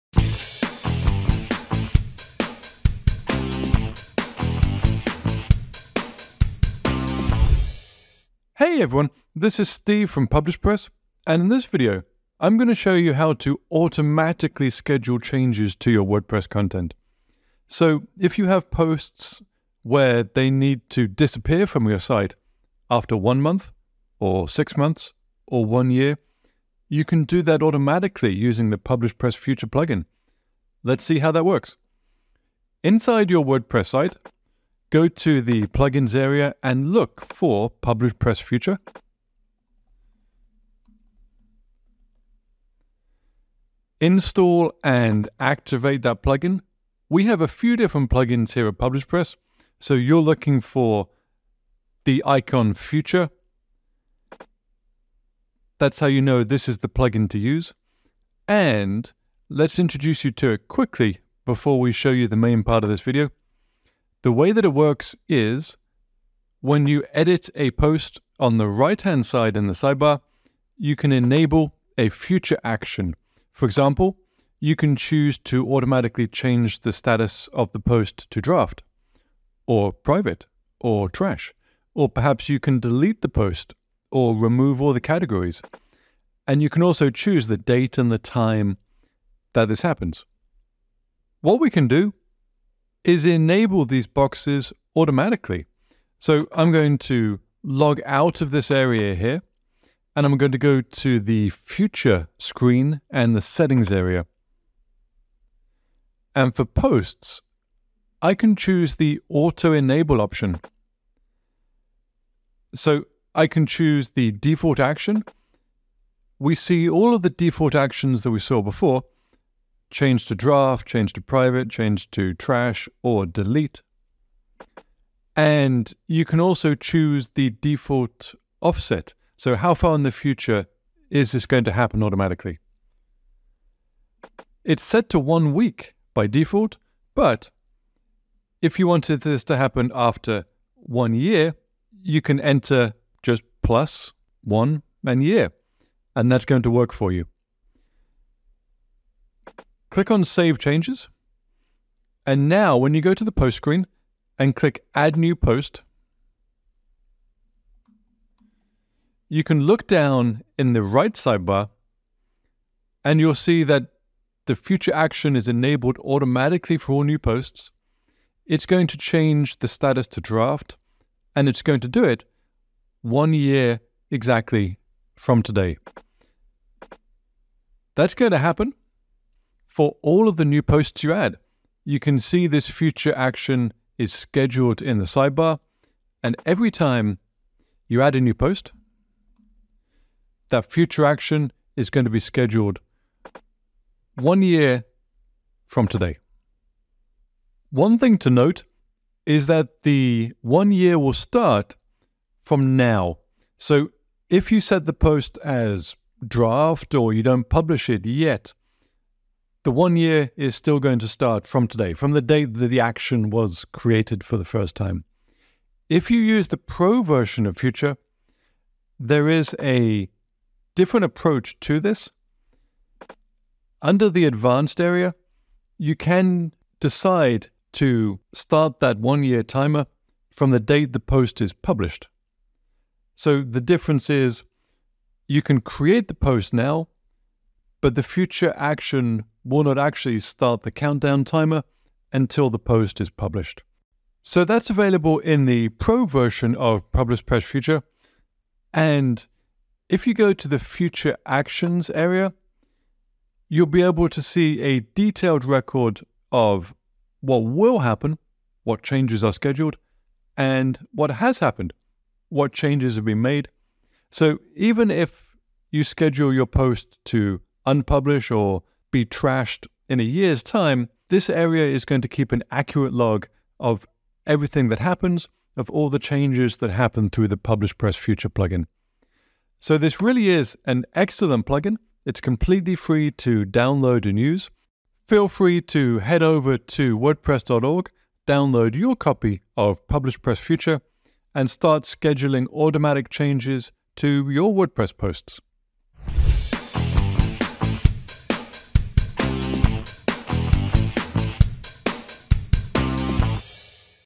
Automatically Schedule Changes to Your WordPress Posts
The PublishPress Future plugin allows you to make automatic changes to posts, pages and other content types. You can use set default expiry options for each post type. For example, whenever you publish a new post, you can automatically set the post to expire after 1 month.